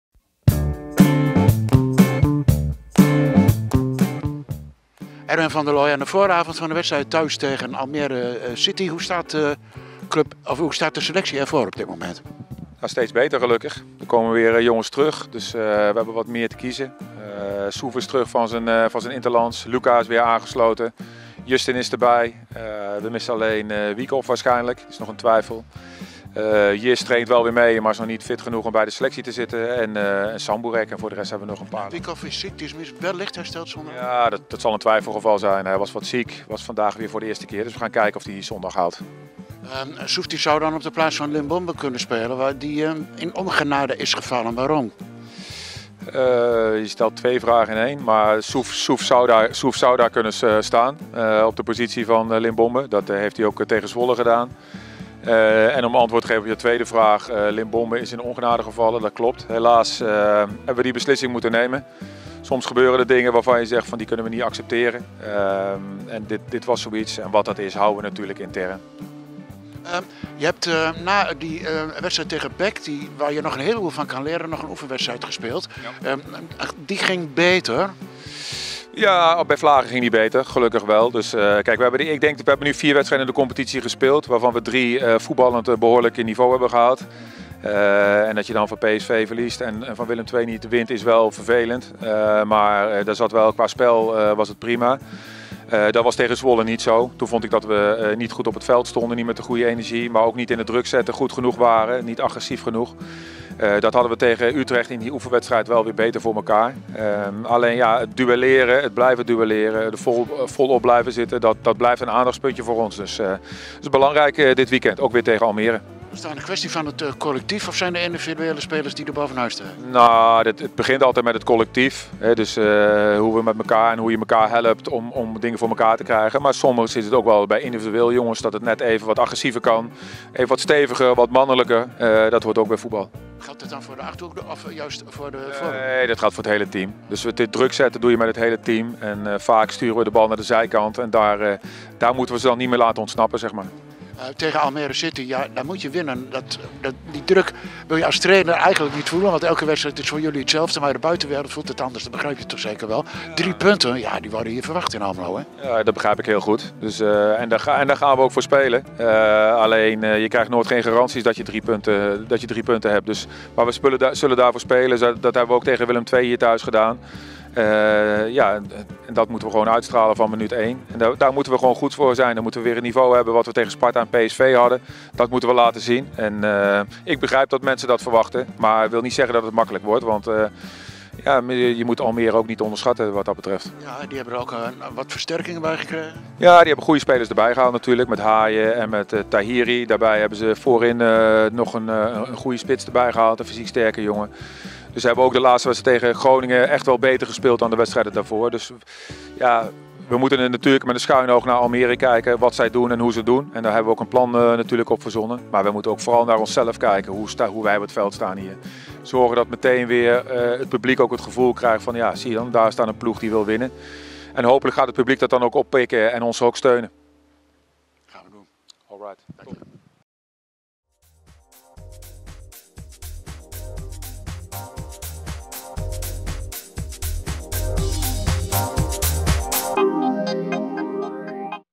Voorbeschouwing Heracles Almelo tegen Almere City
Komend weekend staat speelronde vijf van de Eredivisie op het programma. Heracles Almelo neemt het in eigen huis op tegen Almere City FC. De wedstrijd start zondag om 16.45 uur in het Asito stadion.